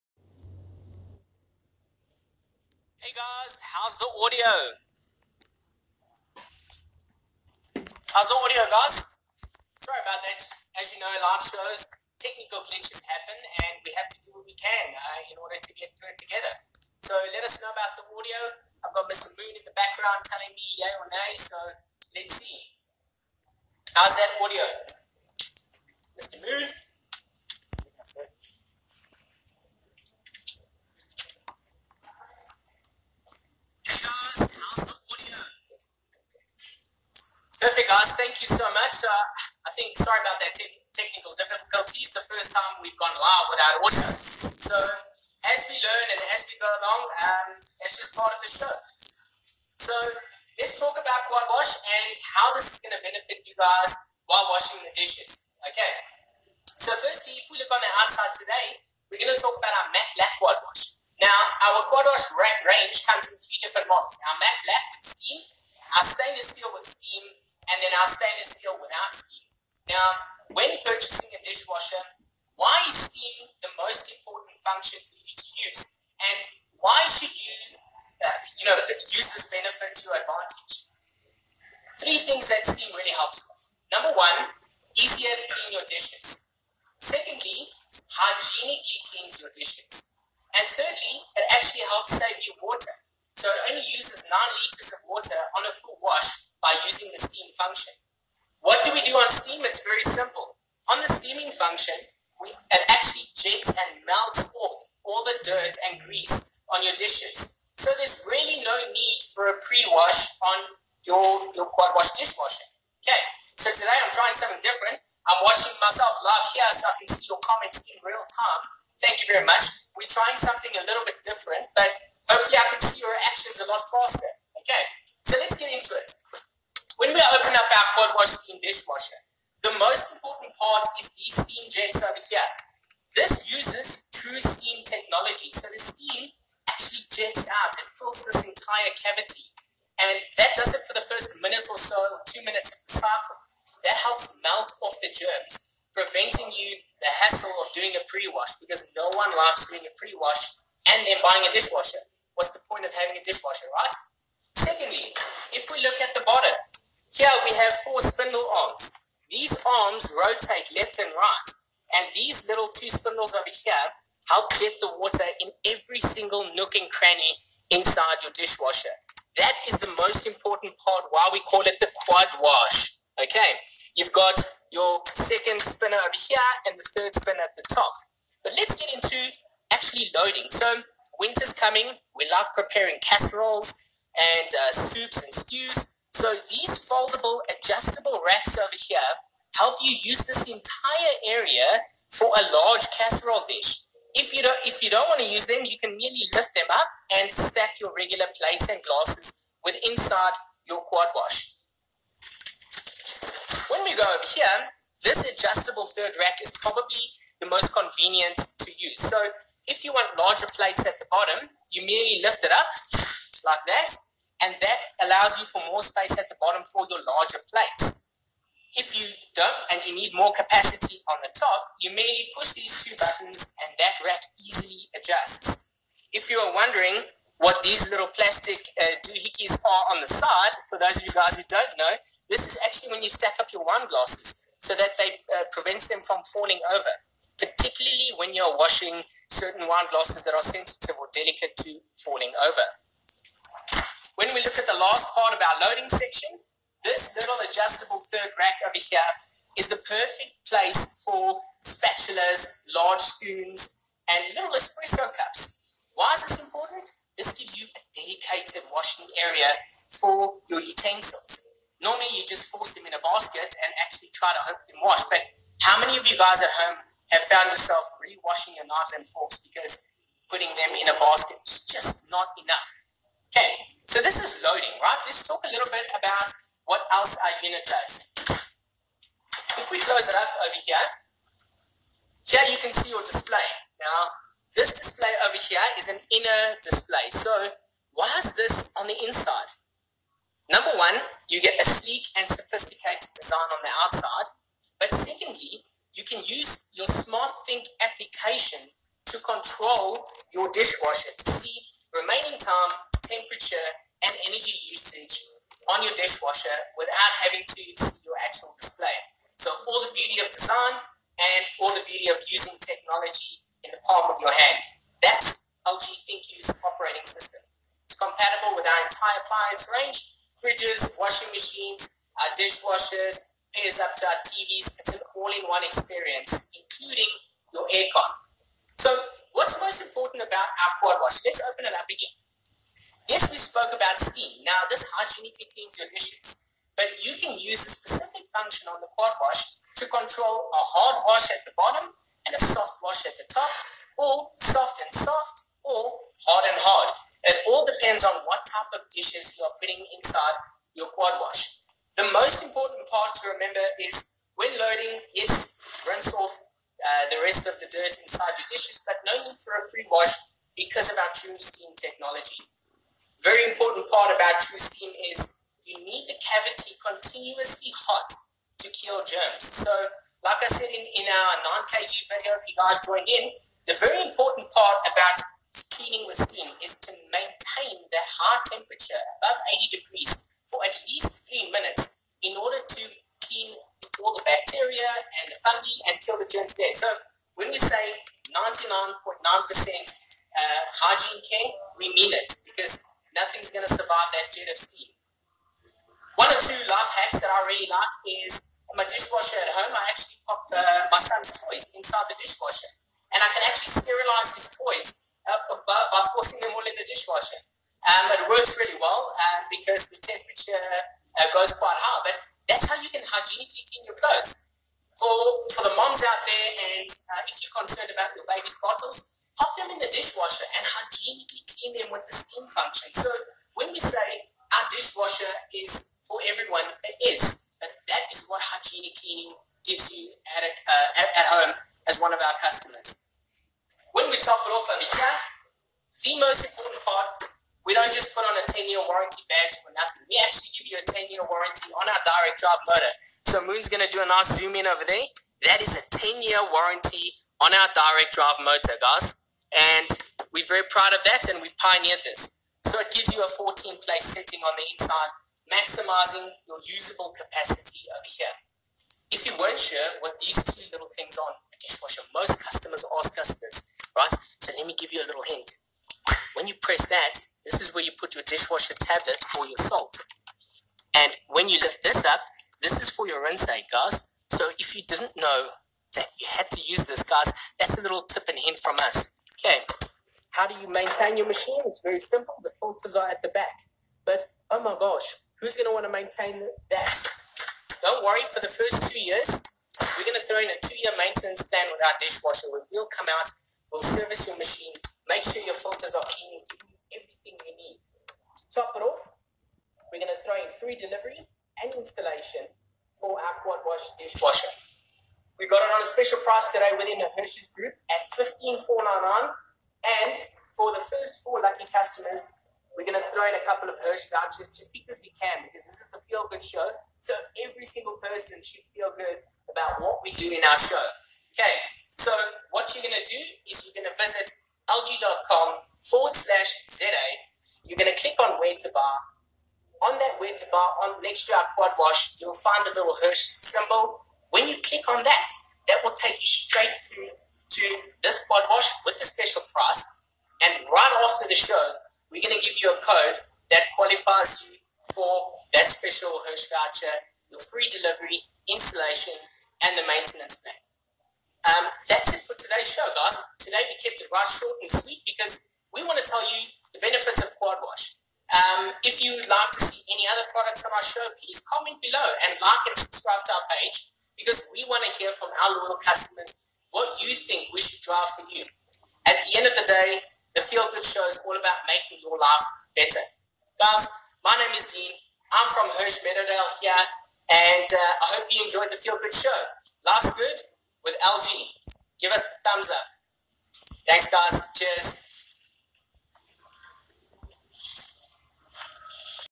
Feel Good Show #3
We talk about Steam in our Dish washer